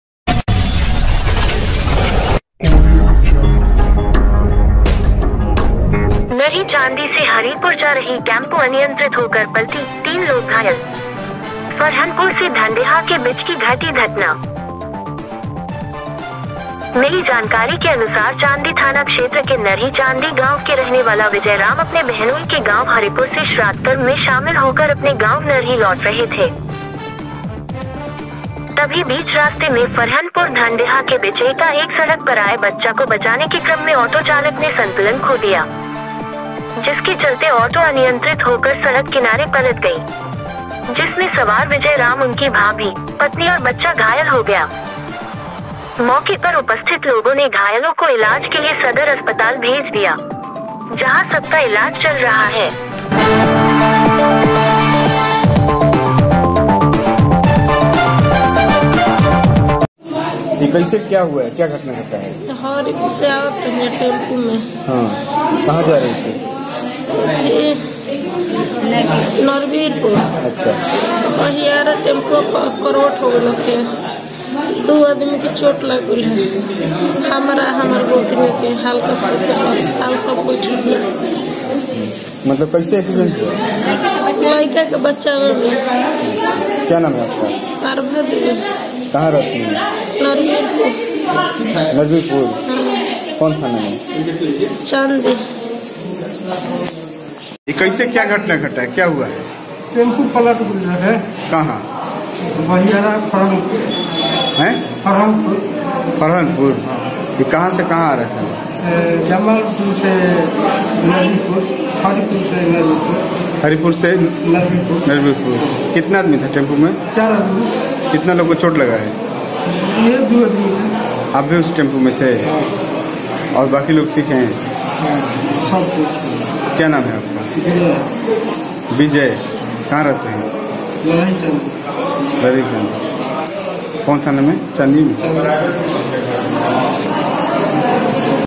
टेंपो हादसे में 3 लोग घायल, इलाज के लिए पहुंचे आरा सदरहॉस्पिटल
नरही चांदी से हरीपुर जा रही टेम्पु अनियंत्रित होकर पलटी, तीन लोग घायल .फरहनपुर से धनडिहा के बिच की घटी धटना . मिली जानकारी के अनुसार चांदी थाना क्षेत्र के नरही चांदी गांव के रहने वाला विजय राम अपने बहनोई के गांव हरिपुर से श्राधकर्म मे शामिल होकर अपने गांव नर ही लौट रहे थे.। तभी बीच रास्ते मे फरहनपुर धनडिहा के बिच एका एक सड़क पर आए बच्चा को बचाने के क्रम मे ऑटो चालक ने संतुलन खो दिया.। जिसके चलते ऑटो अनियंत्रित होकर सड़क किनारे पलट गई।. जिसमे सवार विजय राम उनकी भाभी, पत्नी और बच्चा घायल हो गया। .मौके पर उपस्थित लोगो ने घायलो को इलाज के लिए सदर अस्पताल भेज दिया।. जहा सबका इलाज चल रहा है।.